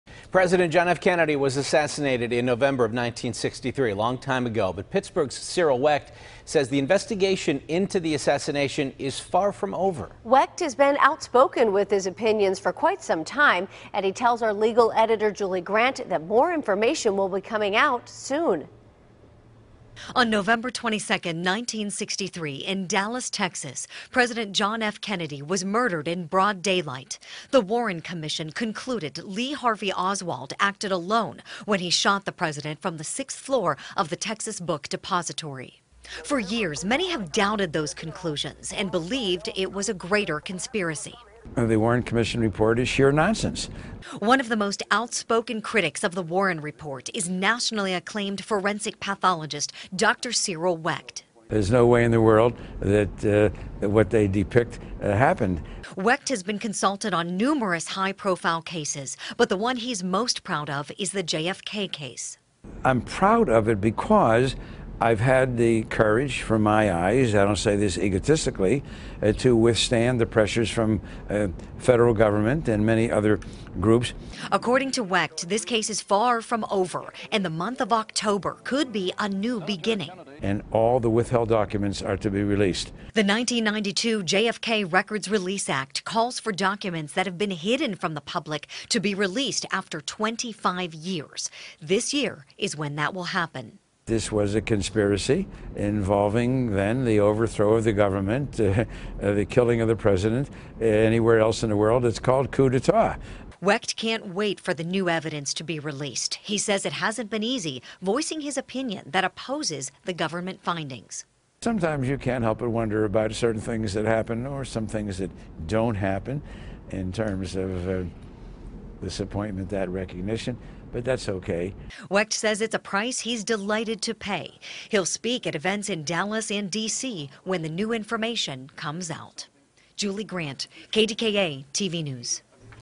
Pittsburgh Pathologist Says JFK Assassination Investigation Is Far From Over
A Pittsburgh forensic pathologist says the investigation into the JFK assassination is far from over; KDKA's Julie Grant reports.